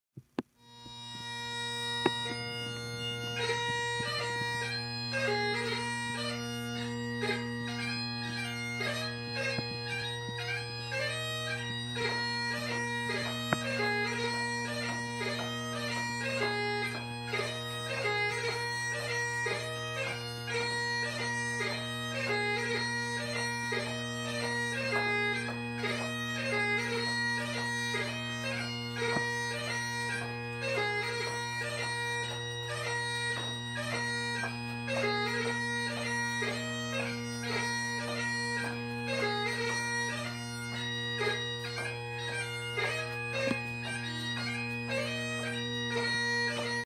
Sword 1 - Addressing The Swords
An aid to practice the first step of the Sword with music.

This video is for practice purposes only and was not made with the intention of it being publicly viewed.